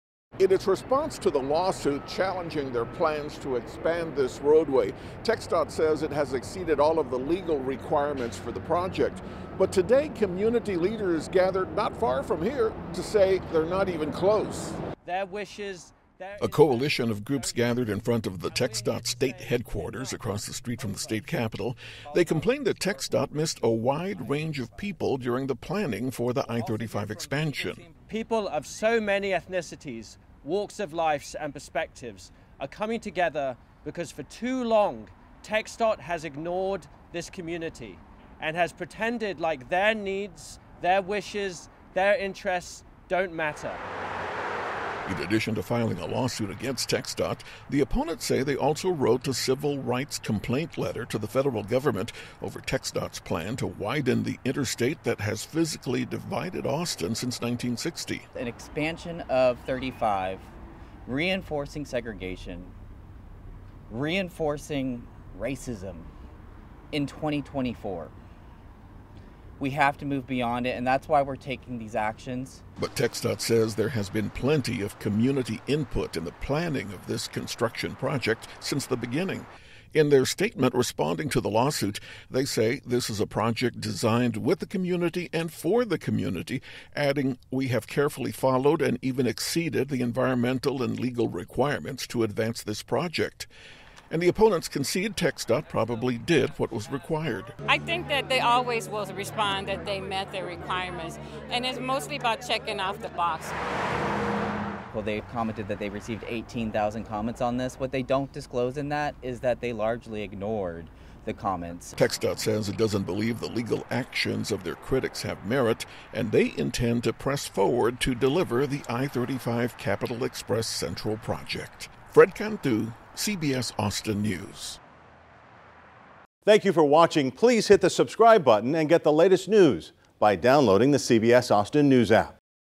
Austin groups challenge I-35 expansion with civil rights complaint
Opponents of the Interstate 35 widening project in central Austin, including a coalition of groups, have filed a lawsuit and a federal civil rights complaint against the Texas Department of Transportation, alleging inadequate community representation and input in the planning process.